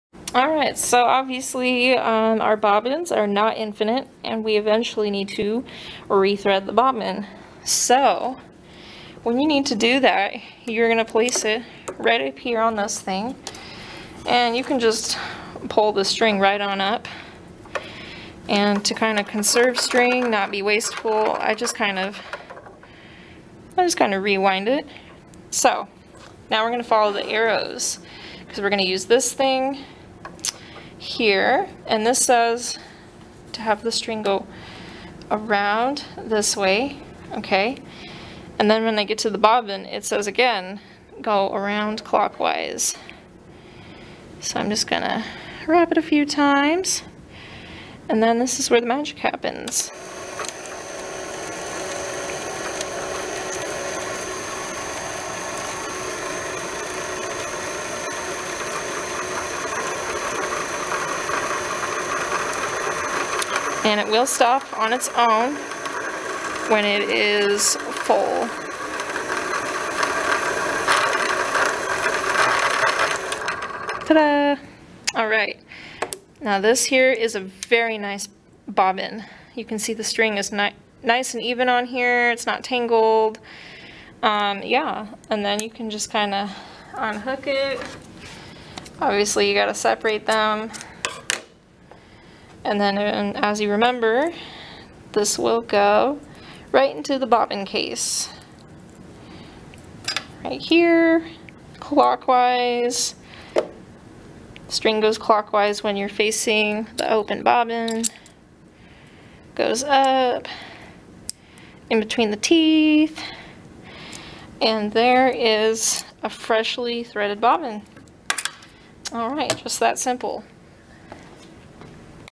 How to Thread a Bobbin for Bernina 530
Tutorial showing how to thread a bobbin for a Bernina 530 sewing machine for Washington State University Apparel, Merchandising, Design, and Textile students in the AMDT 211 beginning apparel assemblage class.